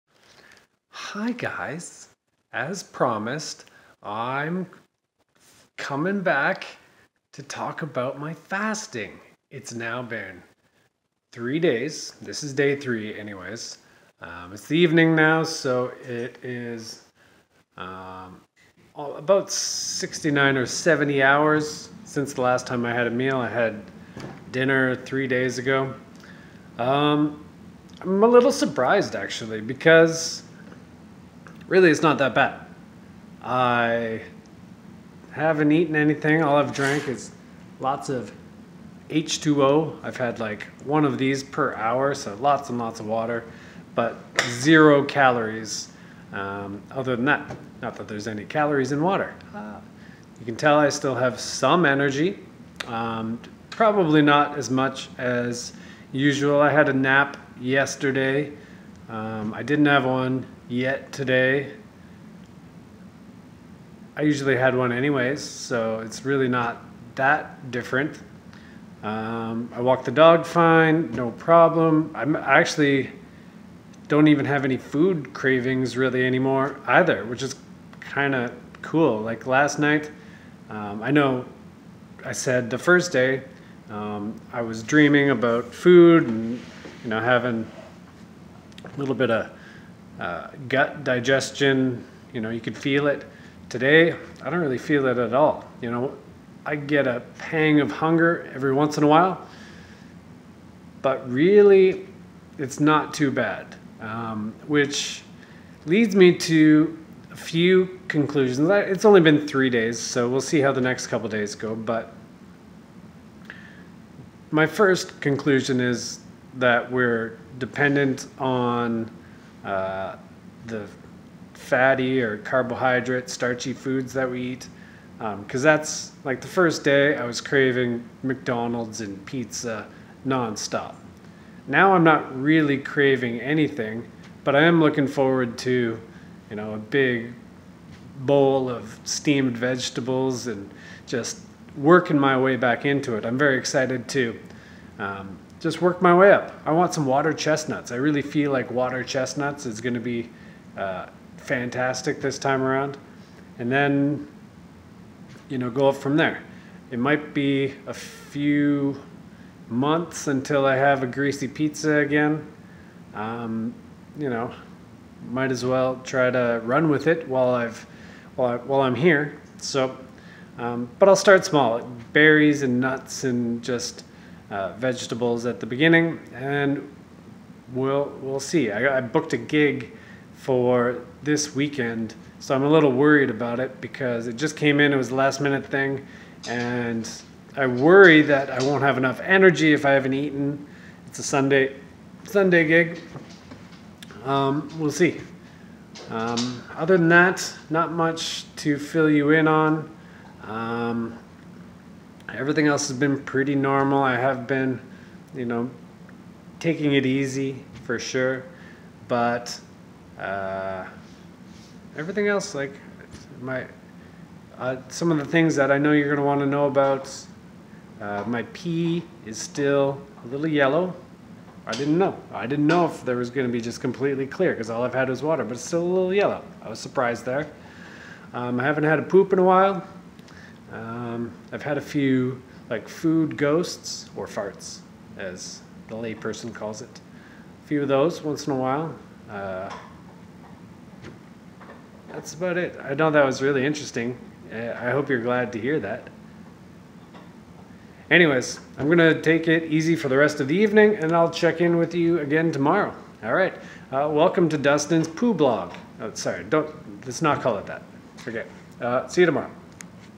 100 Hour Fast: Ketosis Challenge (Day 3)
Haven't eaten in 70 hours. Surprisingly, not too bad!

Join the discussion and talk about fasting for ketosis on our